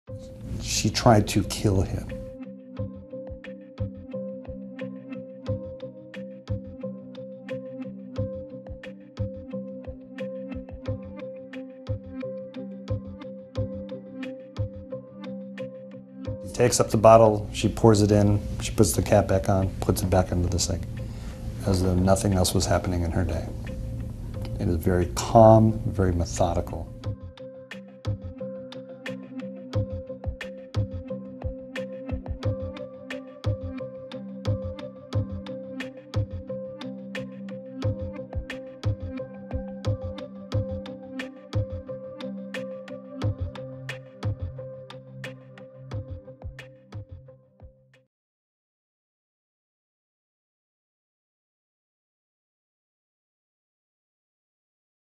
Acusan a una dermatóloga de California de envenenar a su marido con Drano
Una dermatóloga de 45 años fue arrestada por presuntamente envenenar a su esposo con líquido para destapar cañerías. La víctima presentó tres videos grabados con una cámara oculta como prueba. Sigue las últimas noticias en Univision.